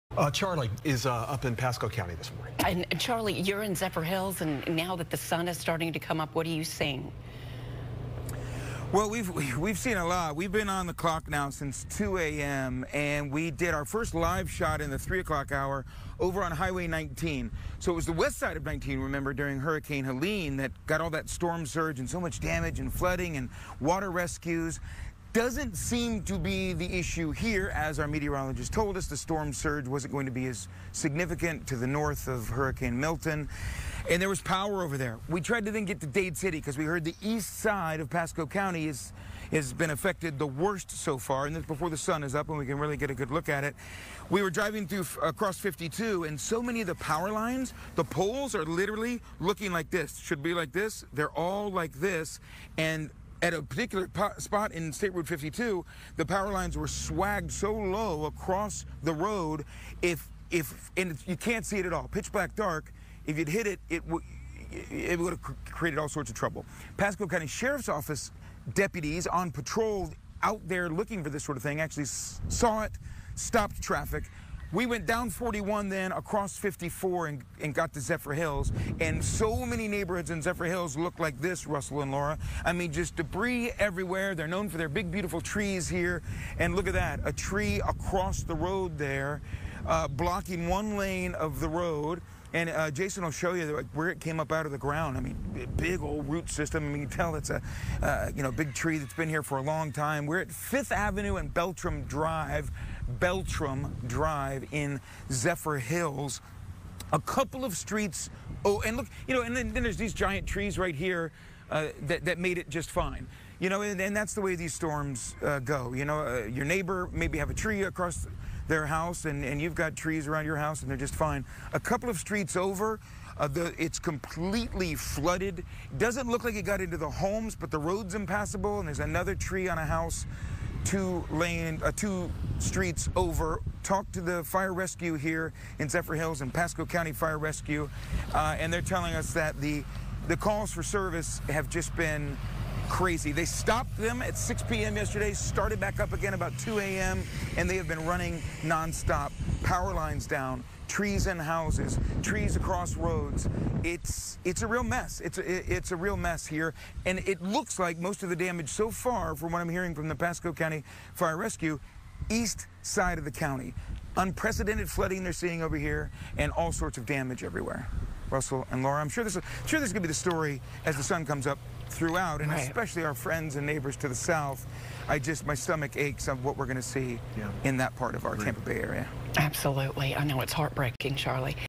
Hurricane Milton's impact on Pasco County
FOX 13's Charley Belcher has the latest on damage caused by Hurricane Milton in Pasco County, including many downed trees in the Zephyrhills area.